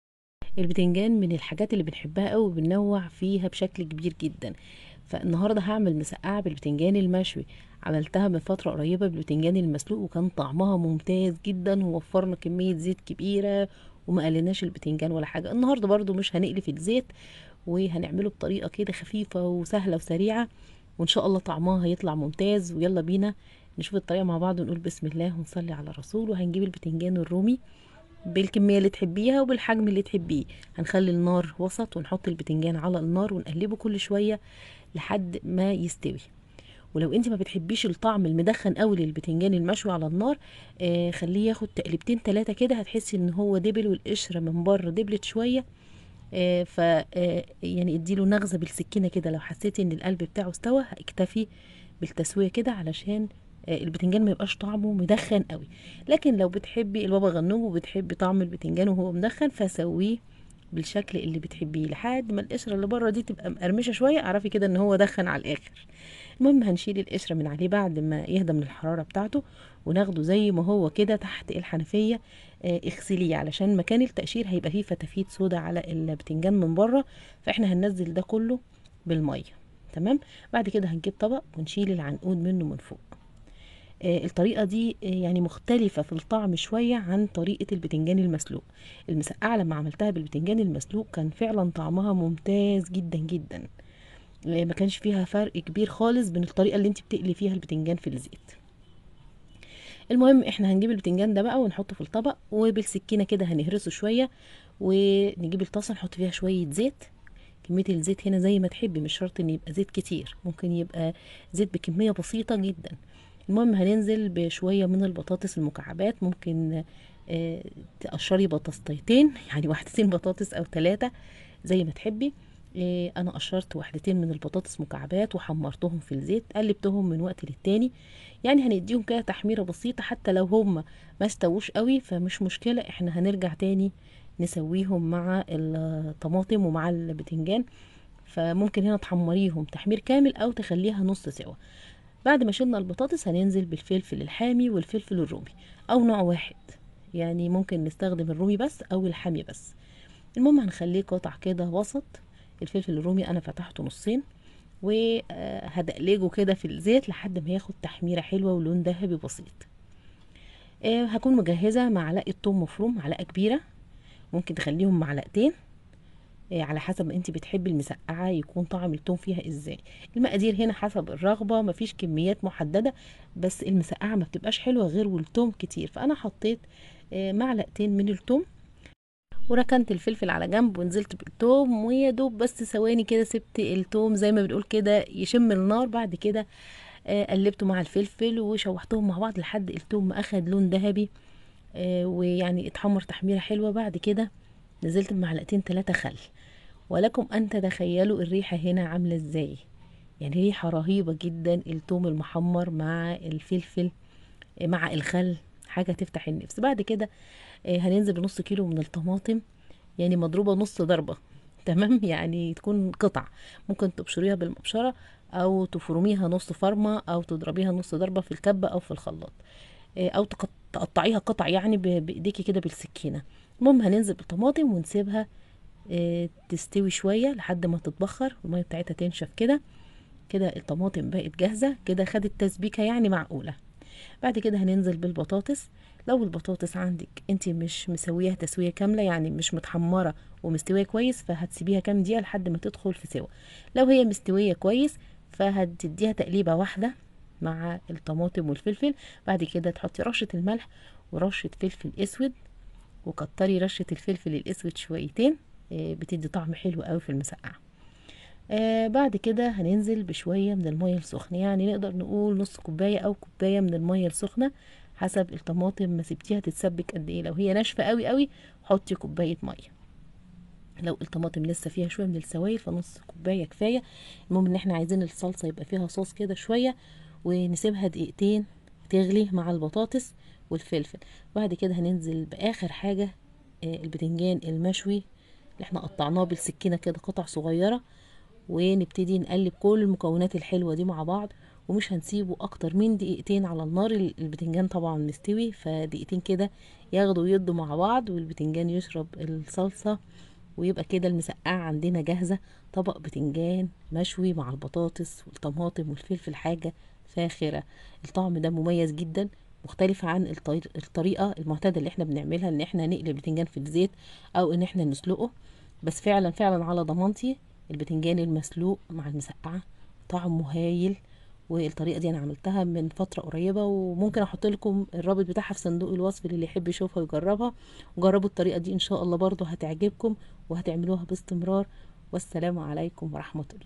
البتنجان من غيرما تسلقية ولا تقلية وتخلصى الزيت علية جربية بالطريقة دى هتاكلى أحلى مسقعة بيه
طريقة لذيذة فى الطعم للمسقعة بالبتنجان المشوى بدون سلق ولا قلى وصفة التوفير والطعم احلى بكتير

المقادير
نصف كيلو بتنجان رومى
٤ ملاعق كبار زيت
٢ بطاطس مكعبات
٤ فلفل حار قطع
٤ فلفل رومى قطع
٢ ملعقة كبيرة ثوم مفروم
نصف كيلو طماطم قطع
نصف ملعقة صغيرة ملح
نصف ملعقة صغيرة فلفل اسود

صفحتنا على الفيسبوك :
هويدا محمود - أكلنا بالمصري

صفحتنا على الإنستجرام :
aklena belmasry